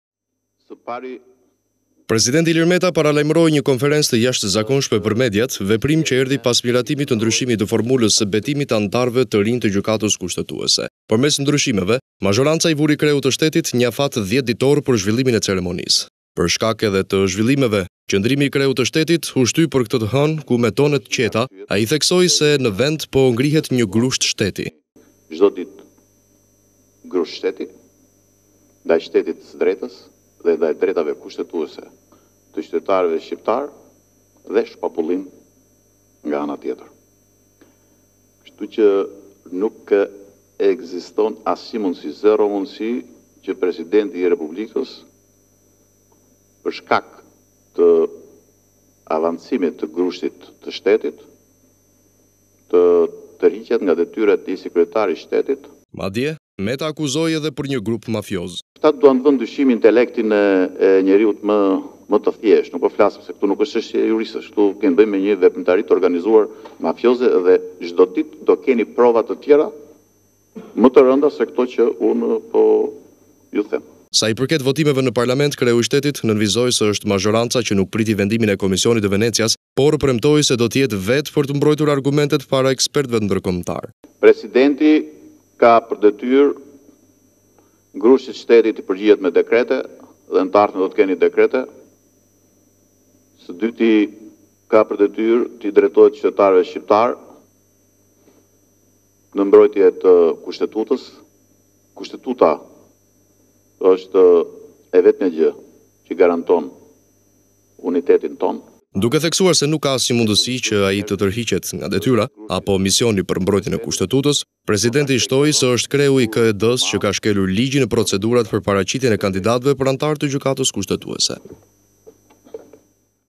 Ora News - Meta: Zero mundësi të tërhiqem për shkak të avancimit të grushtit të shtetit
Presidenti Ilir Meta paralajmëroi një konferencë të jashtëzakonshme për mediat, veprim që erdhi pas miratimit të ndryshimit të formulës së betimit të anëtarëve të rinj të Gjykatës Kushtetuese. Përmes ndryshimeve mazhoranca i vuri kreut të shtetit një afat 10 ditor për zhvillimin e ceremonisë.

Për shkak dhe të zhvillimeve qëndrimi i Kreut të Shtetit u shty për këtë të hënë ku me tone të qeta ai theksoi se në vend po ngrihet një grusht shteti

Ilir Meta: Çdo ditë grusht shteti ndaj shteti të së drejtës dhe ndaj të drejtave kushtetuese të qytetarëve  shqiptare dhe shpopullim nga ana tjetër. Kështu që nuk ekziston asnjë mundësi, zero mundësi që Presidenti për shkak të avancimit të grushtit të shtetit të tërhiqet nga detyra si kryetar i shtetit.

Madje meta akuzoi dhe për një grup mafioz.

Ilir Meta: Këta duan të vënë në dyshim intelektin e njeriut më të thjeshtë. Nuk është çështje juristësh, këtu kemi me një veprimtari të organizuar mafioze dhe çdo ditë do keni prova më të rënda se këto që unë po them.

Sa i përket votimeve në parlament kreu I shtetit nënvizoi se është mazhoranca që nuk priti vendimin e komisionit të Venecias, por premtoi se do të jetë vetë për të mbrojtur argumentet para ekspertëve ndërkombëtare

Ilir Meta: Presidenti ka për detyrë që grushtit të shtetit ti përgjigjet me dekrete. Ka për detyrë ti drejtohet qytetarëve shqiptare në mbrojtje të Kushtetutës. Kushtetuta është e vetmja gjë që garanton unitetin tonë, pavarësisht se dikush është nga jugu dikush nga veriu, dikush është i krishterë dhe dikush mysliman, dikush është socialist dhe dikush demokrat, dikush ballist e dikush demokrat. Por e vetmja gjë që na bashkon në këtë shtet është vetëm kushtetuta.